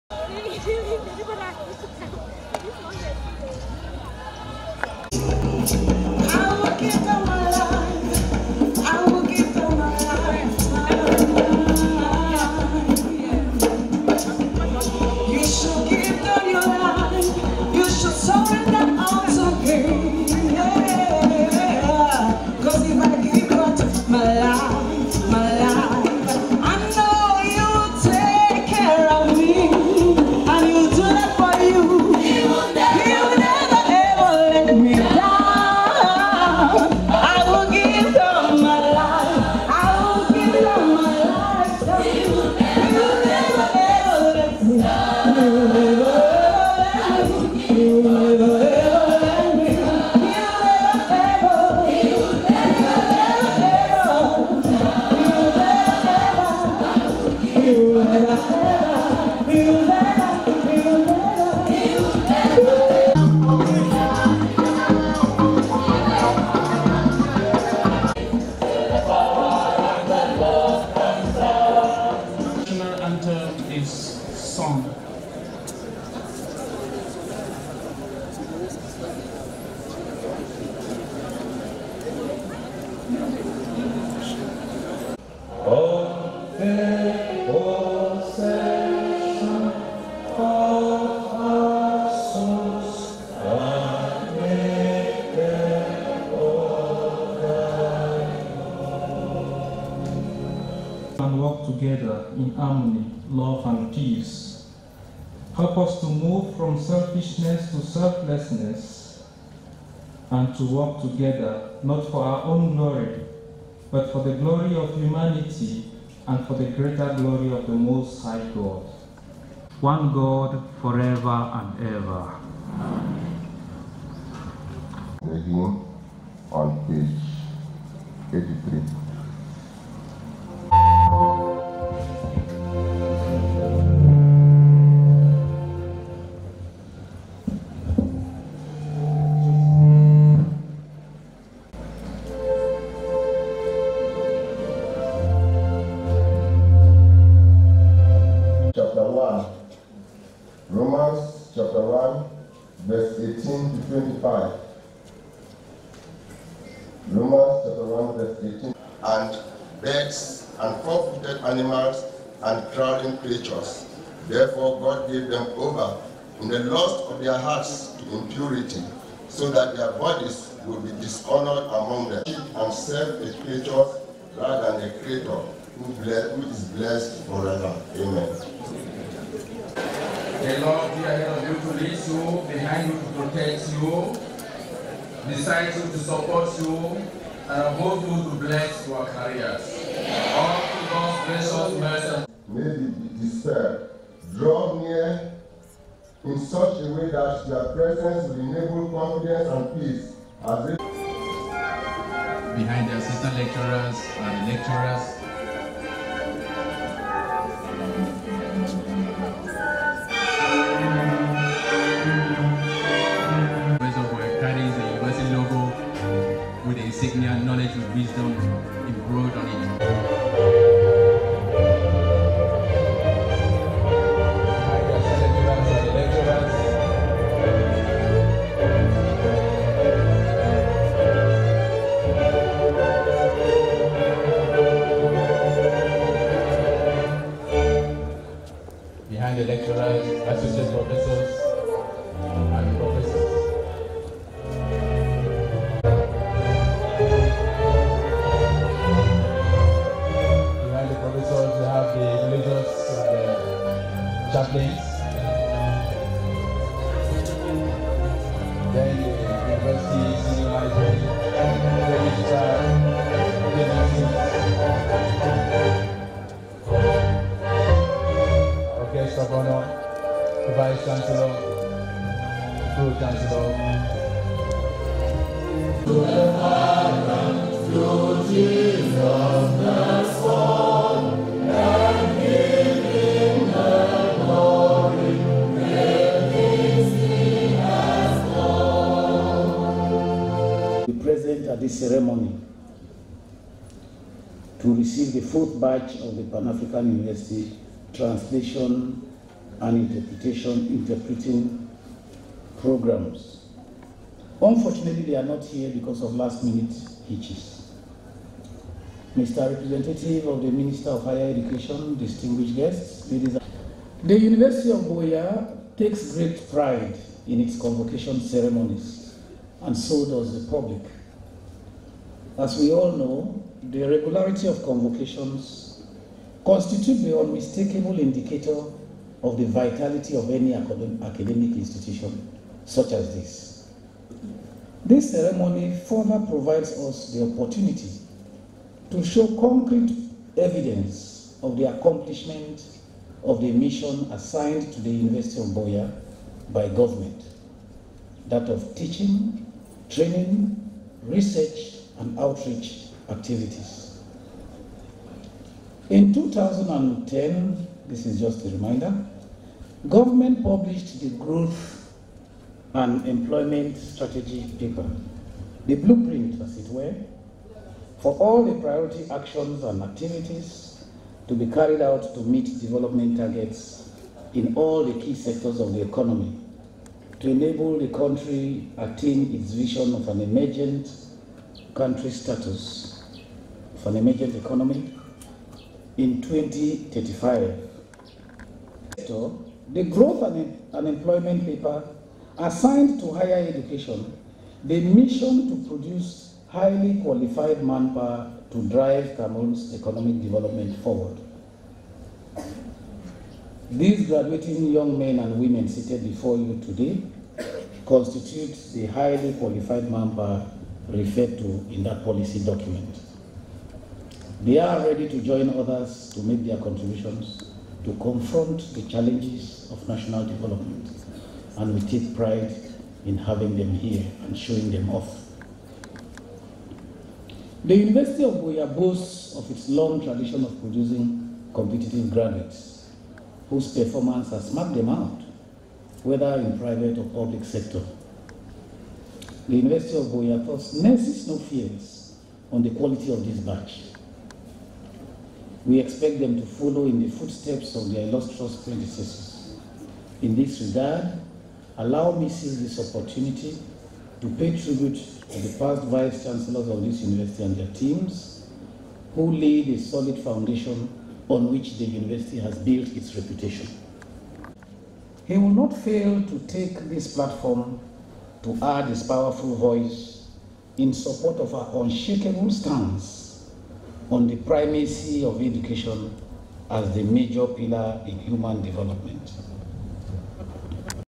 University of Buea 2018 Convocation ceremnony
2018 Convocation ceremony held at the Herbert Nganjo Endeley Amphitheater today 12th December 2018 at the University of Buea campus.